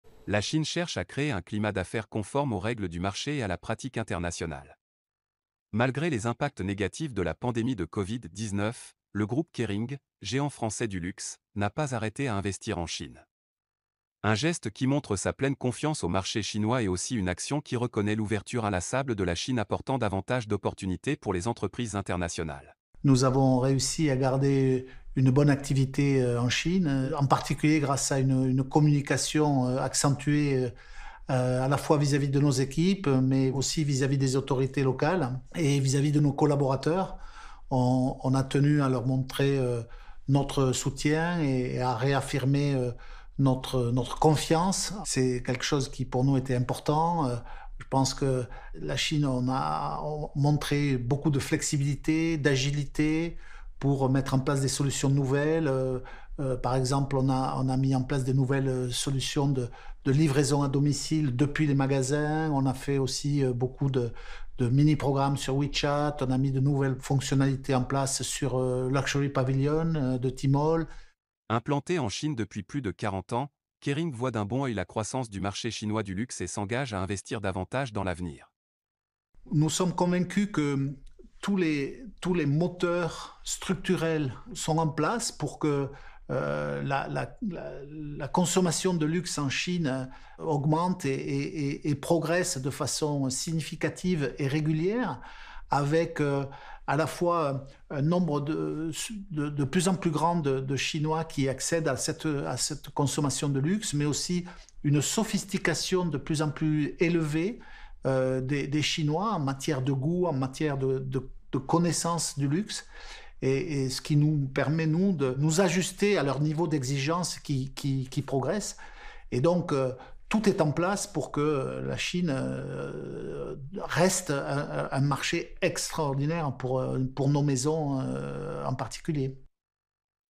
Le marché chinois : « un grand avenir » pour le groupe du luxe Kering de France
La Chine s’engage à ouvrir davantage son marché et à faire profiter au monde entier de son développement. Et les investisseurs étrangers dans le pays en sont témoins.